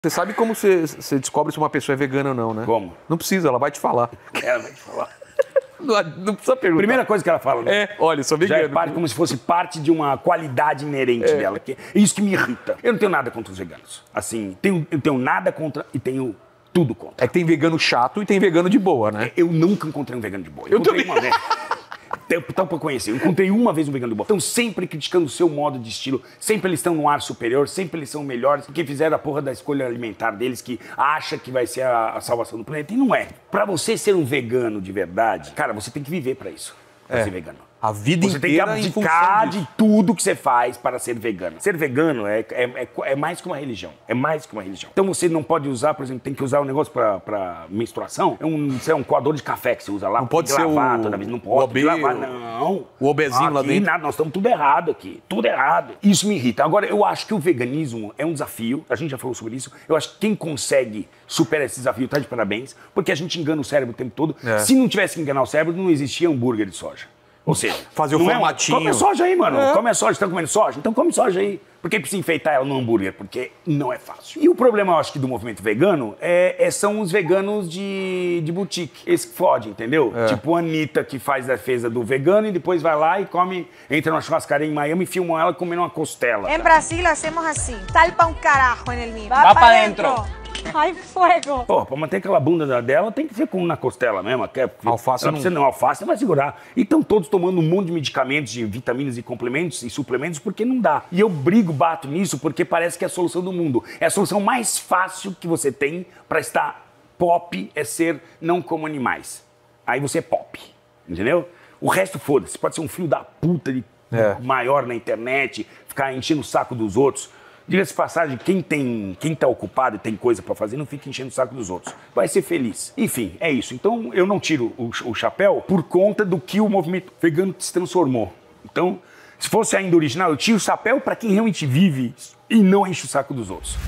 A VERDADE SOBRE O VEGANISMO | Cortes Kiviral
Os melhores cortes de podcasts estão aqui! DUVIDA? Vem ver

Vídeo original: RICHARD RASMUSSEN DÁ SUA OPINIÃO SOBRE VEGANOS e até mesmo a ANITTA!